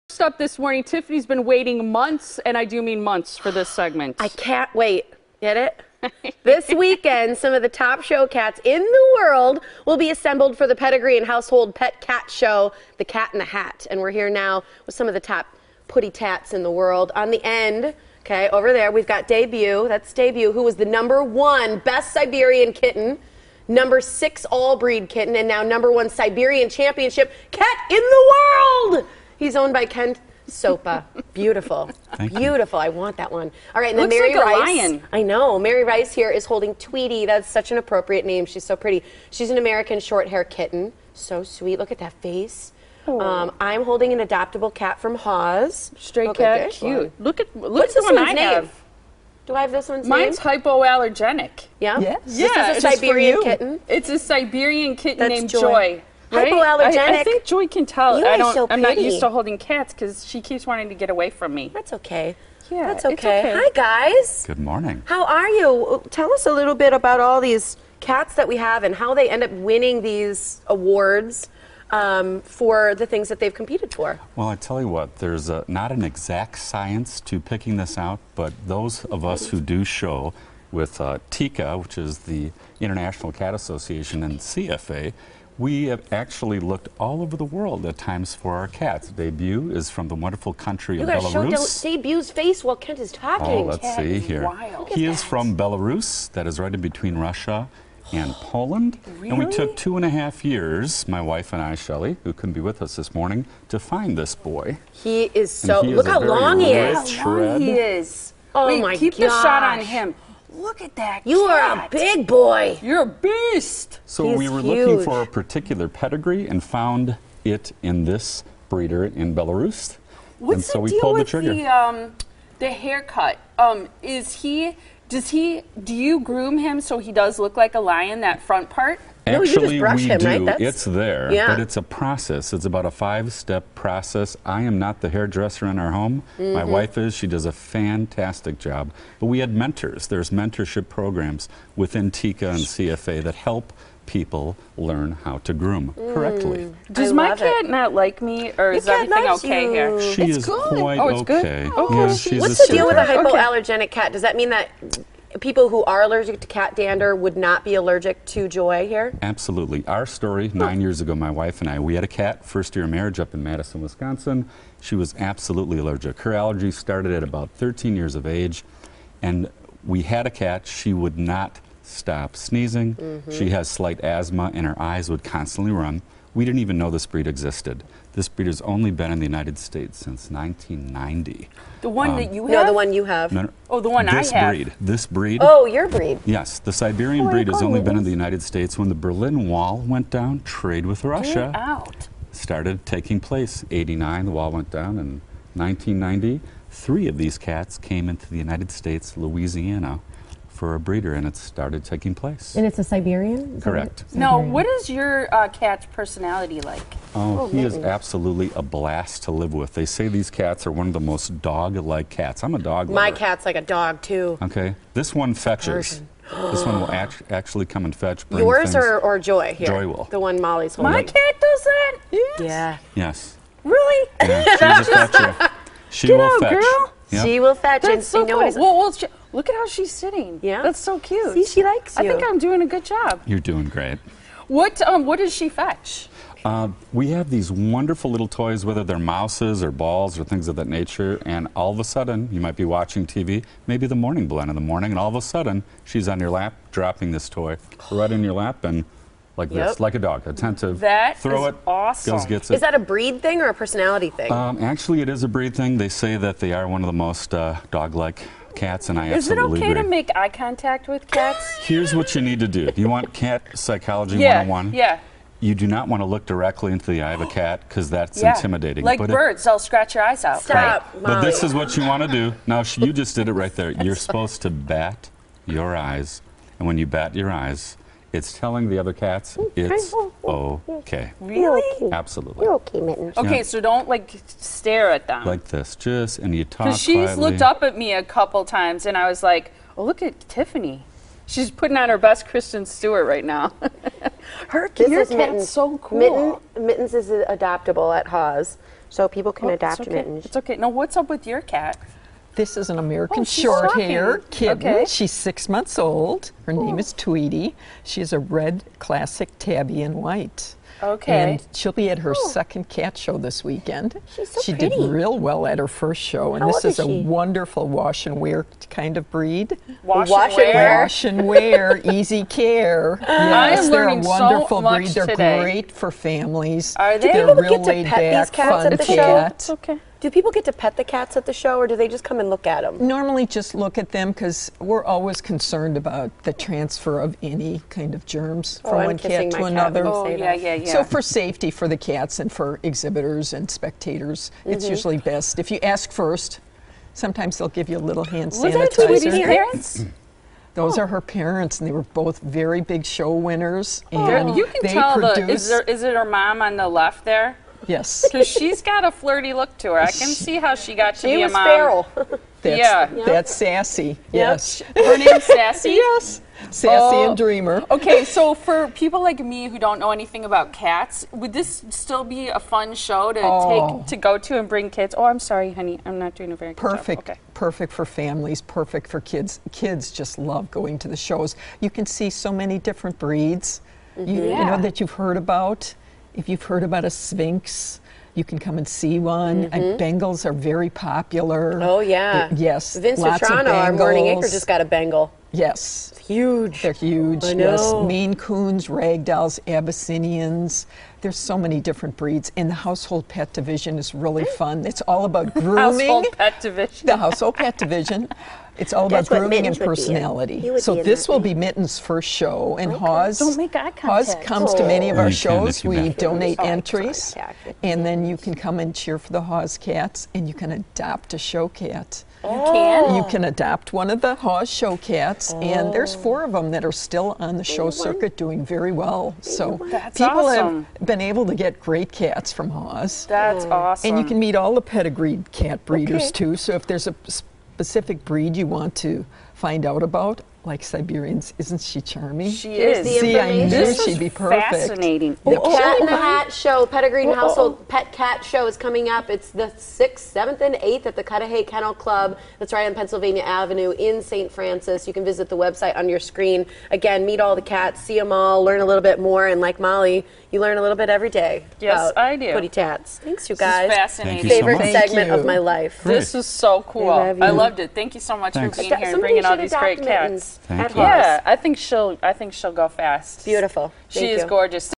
A One-of-a-Kind Cat Show
We have the PURR-fect event for you! The Wisconsin-Illinois Cat Fanciers present their 29th year hosting cat shows with their 4th Annual "The Cat in the Hat" Pedigreed & Household Pet Cat Show. The show features some of the top show cats in the world! HAWS will be showing adoptable kittens and cats. We are joined by Liz Hansen, Kent Sopa, and Mary Reiss to discuss the event. The Cat Show takes place from March 6 -8 at Cudahy Kennel Club.